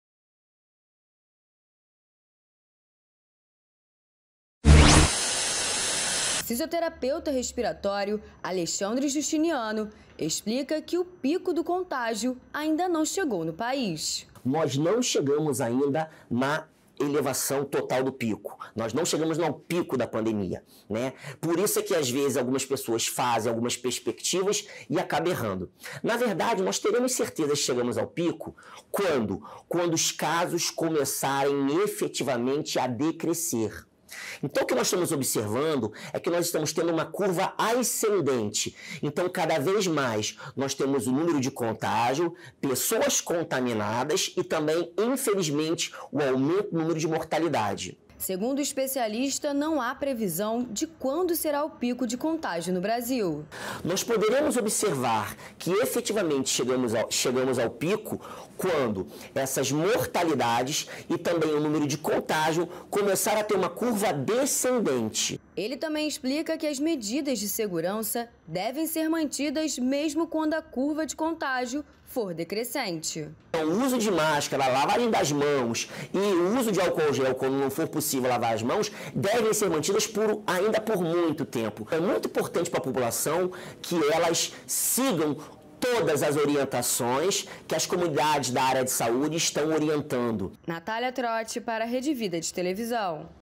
Saiu na Mídia: Membro da CT de Fisioterapia Intensiva dá entrevista à Rede Vida.
Dr. Alexandre Justiniano, membro da Câmera Técnica de Fisioterapia Intensiva, dá entrevista à Rede Vida e fala sobre a curva de contágio da COVID-19 não ter chegado ao pico.